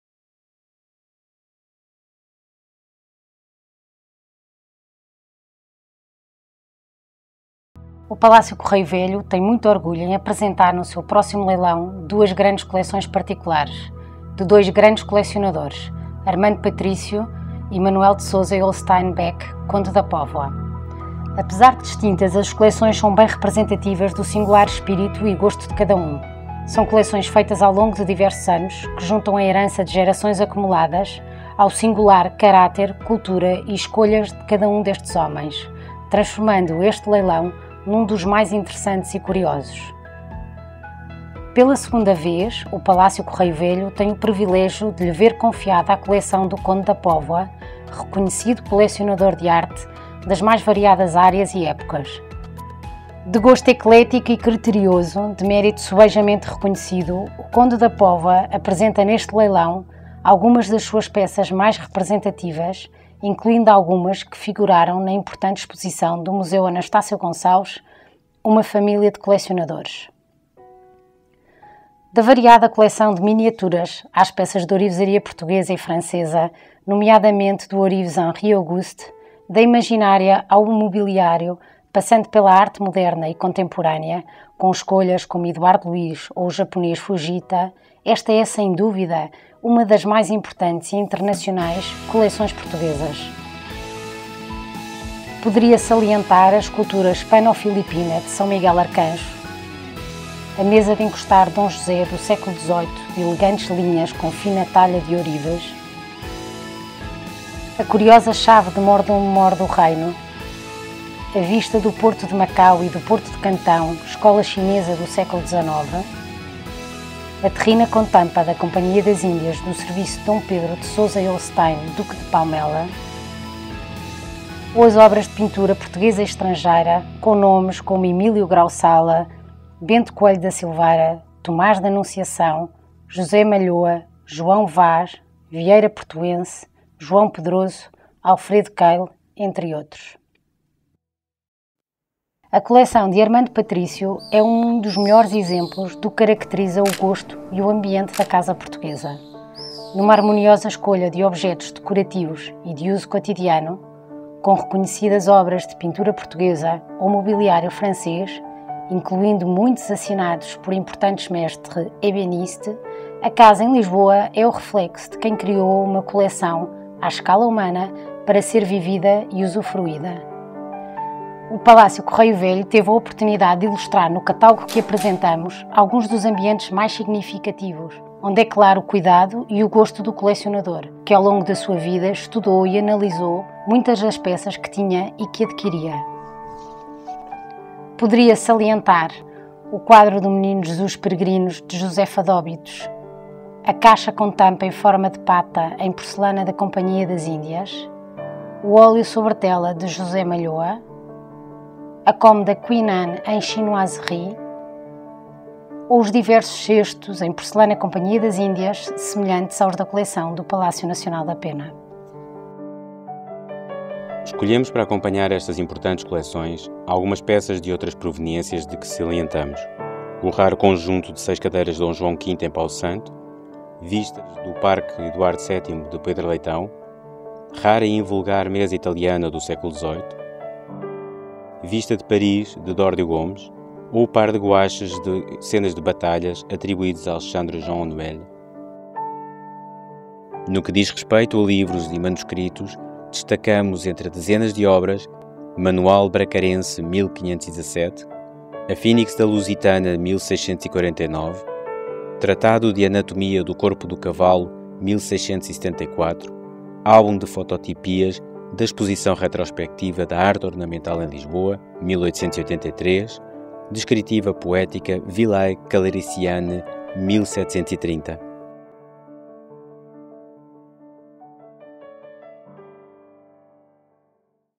Apresentação dos Leilões da Rentrée de Setembro
Palácio do Correio Velho

Apresentação do próximos Leilãos de Antiguidades com Exposição conjunta.

Exposição L309 e L311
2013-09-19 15:00 / 19:00 - 21:00 / 23:00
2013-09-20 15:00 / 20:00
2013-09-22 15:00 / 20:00

Leilão 309
Colecção D. Manuel de Souza e Holstein Beck, Conde da Póvoa
II Parte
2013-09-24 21:00 | Lote 0001 a 0212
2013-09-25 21:00 | Lote 0213 a 0406

Leilão 311
Colecção Armando Patrício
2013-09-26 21:00 | Lote 0407 a 0614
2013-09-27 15:00 | Lote 0615 a 0784